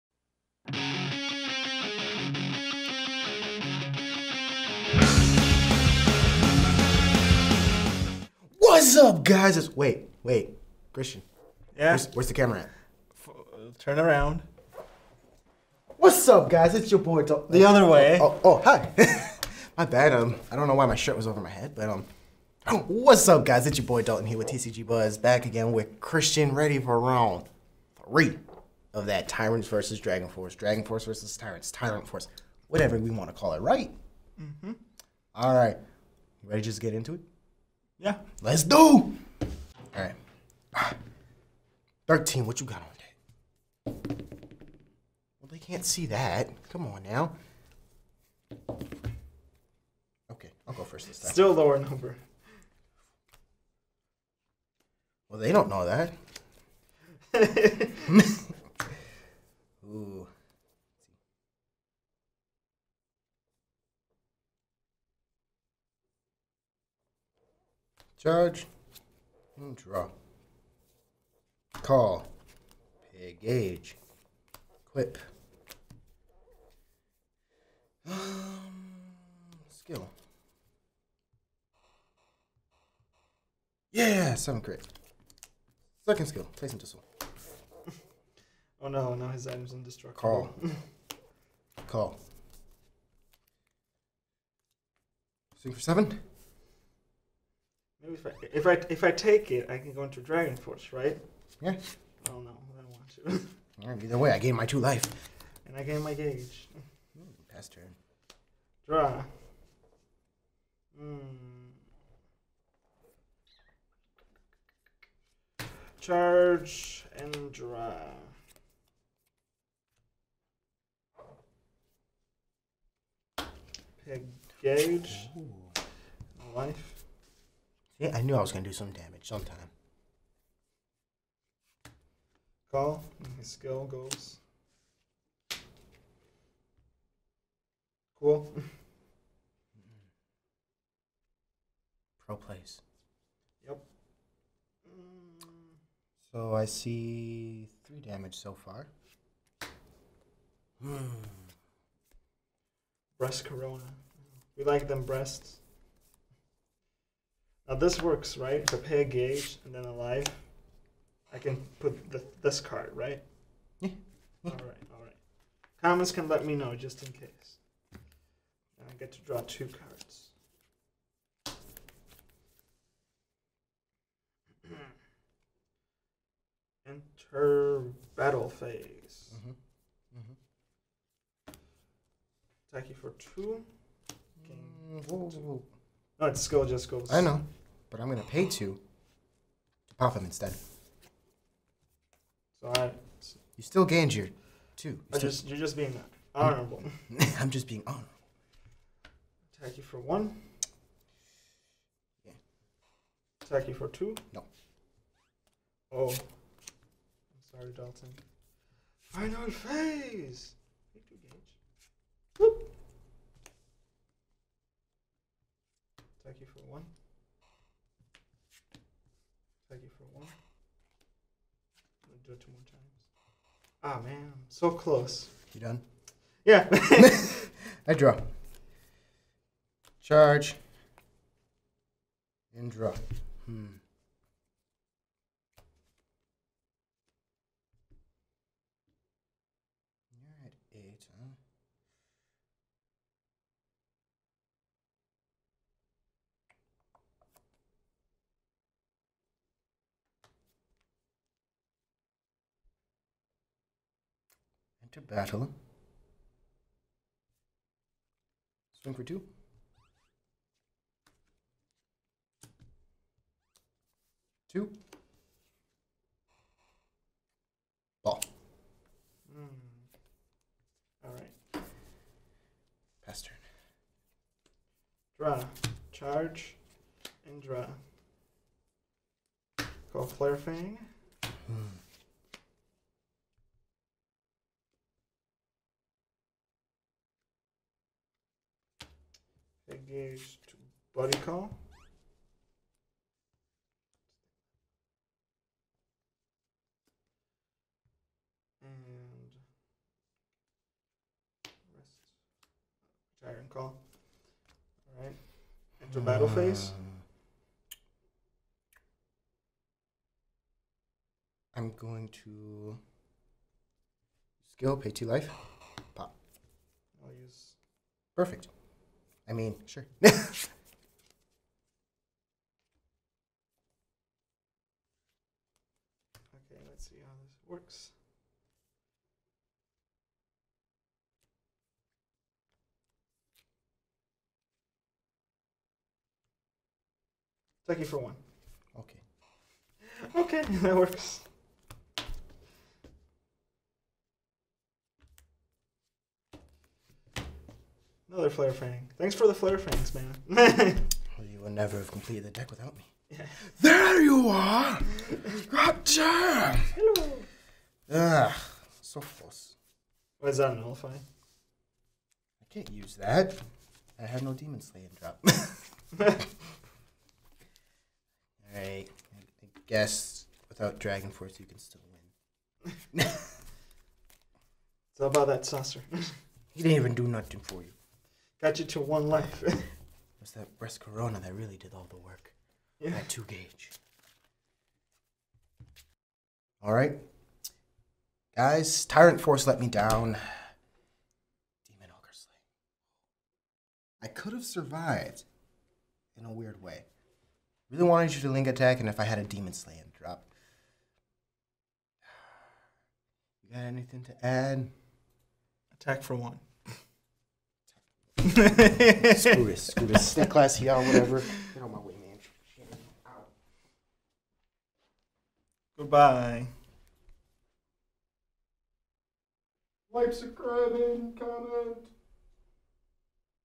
Buddyfight Match! Sun Dragon Force vs Tyrants Match 3
Its time to release the Dragon Force! In today's Match we have Sun Dragons vs Tyrants, a very epic battle between two aggro decks

Let us know what match-ups you want to see next!